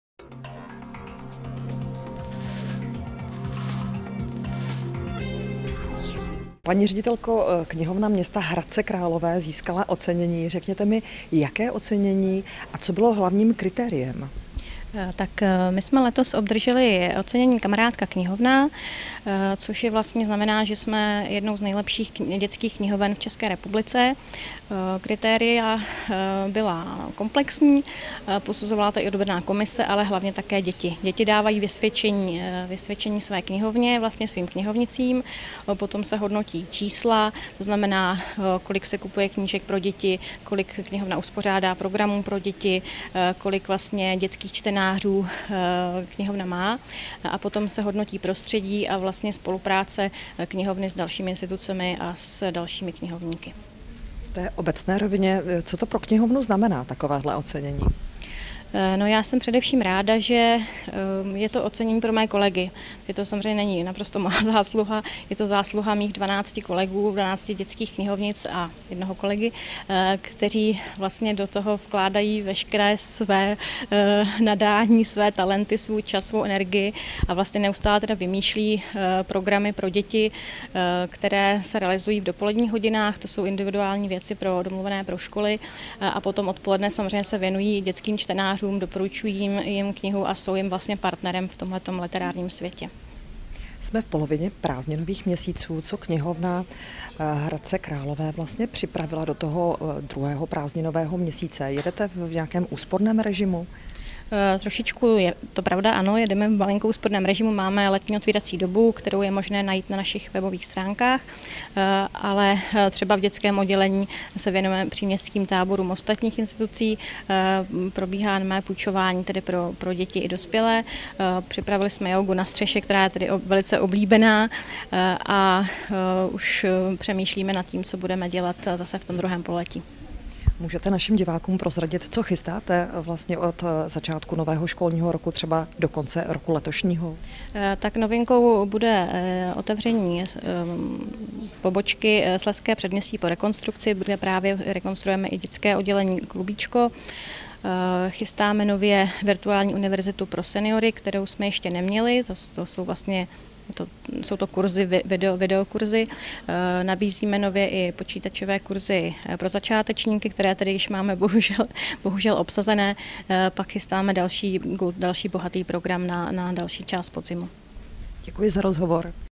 Oceněná knihovna chystá řadu novinek
Knihovna města Hradce Králové letos získala nejvyšší ocenění pro děti v soutěži Kamarádka knihovna. Nejen o ocenění, ale také o plánech do následujících měsíců jsme hovořili s ředitelkou Knihovny města Hradce Králové Barborou Čižinskou.